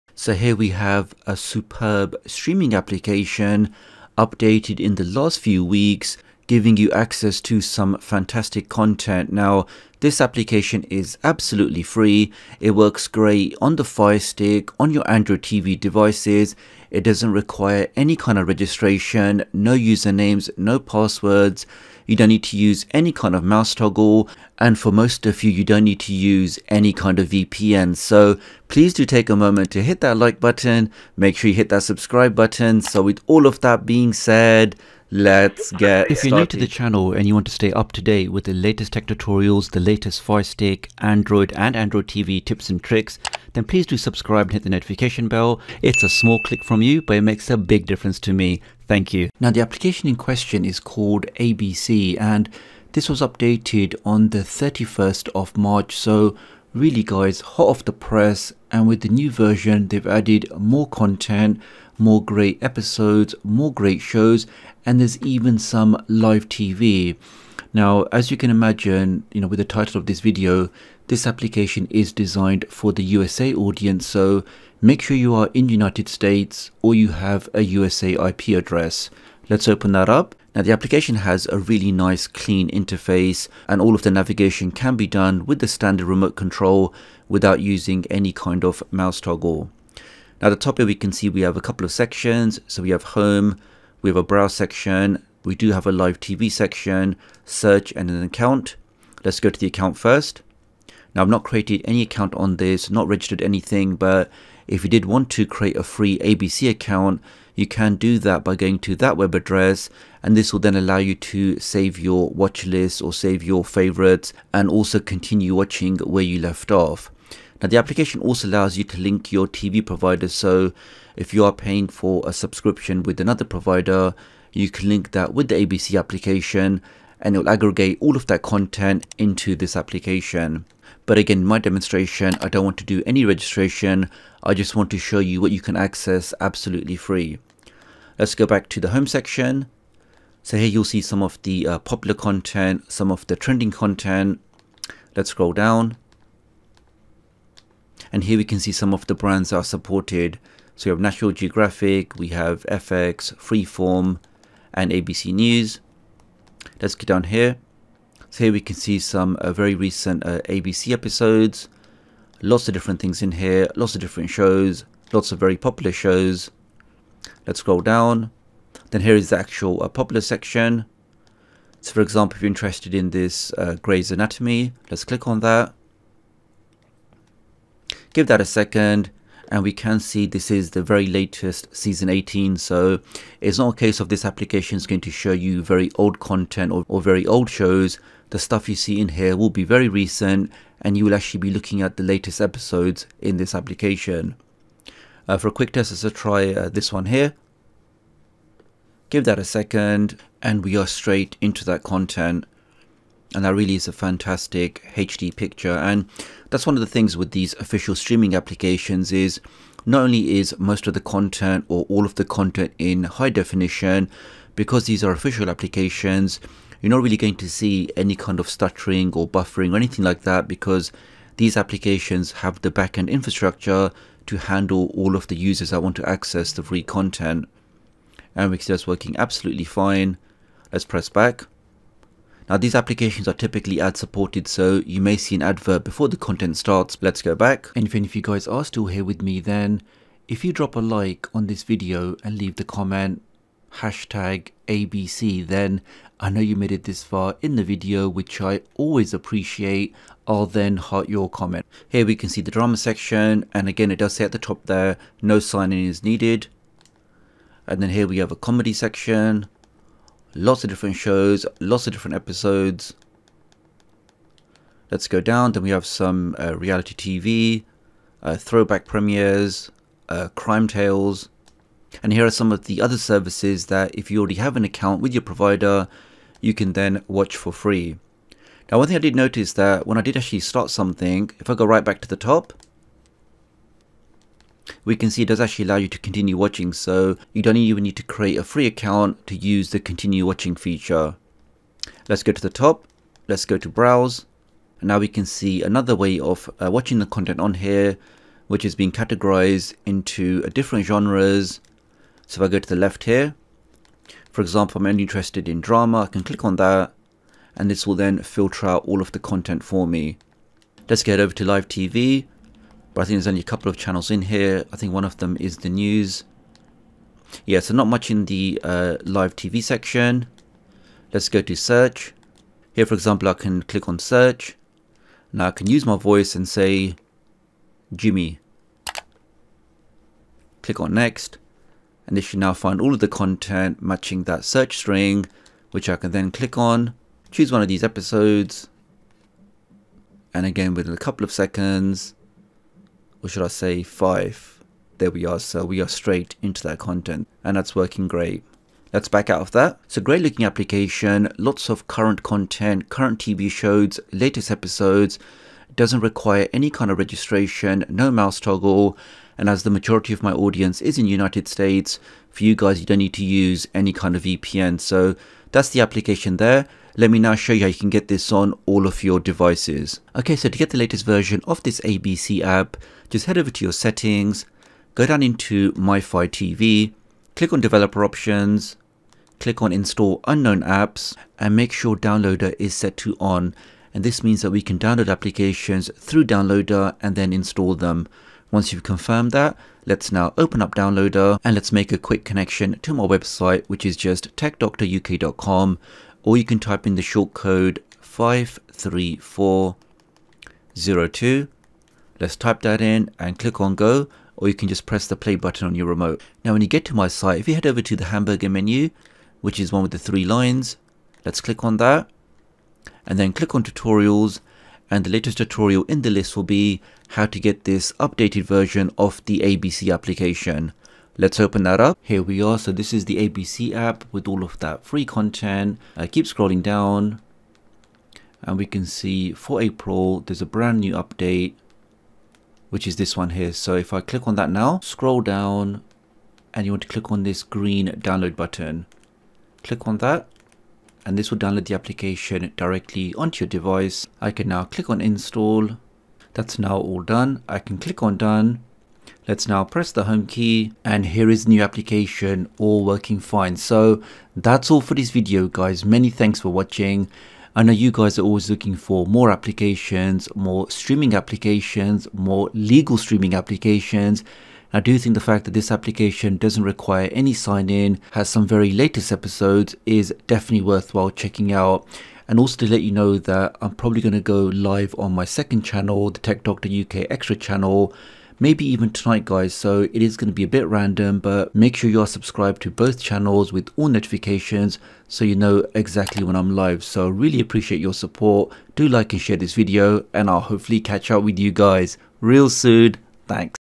🔴GREAT USA STREAMING APP (2022 CONTENT) !
A new streaming application with VOD content, works great on Firestick and Android TV devices. Application is available direct on the Amazon and Google play stores.